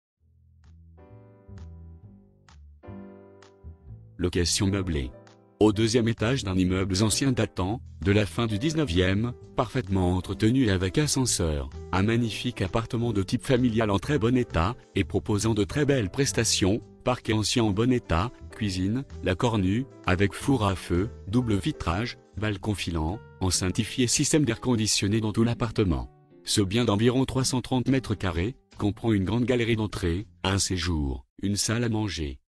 Magnifique appartement familial avec 5 chambres + bureau - Bosquet - Paris 7ème
LOCATION MEUBLÉE. Au deuxième étage d'un immeuble ancien datant de la fin du XIXe, parfaitement entretenu et avec ascenseur, un magnifique appartement de type familiale en très bonne état et proposant de très belles prestations (parquet ancien en bon état, cuisine 'Lacornue' avec four à feu, double vitrage, balcon filant, enceintes HIFI et système d'air conditionné dans tout l'appartement...). Ce bien d'environ 330m² comprend une grande galerie d'entrée, un séjour, une salle à manger, une grande cuisine dînatoire équipée Lacornue), un bureau ou salle TV. Une suite parentale avec deux dressings de chaque coté. Une grande salle de bains avec baignoire et cabine de douche séparée. Deux chambres avec leur salles d'eau attenantes, une salle de bains avec toilettes et une buanderie. Sur le palier de Service, 2 chambres de service. Une cave.Très rare.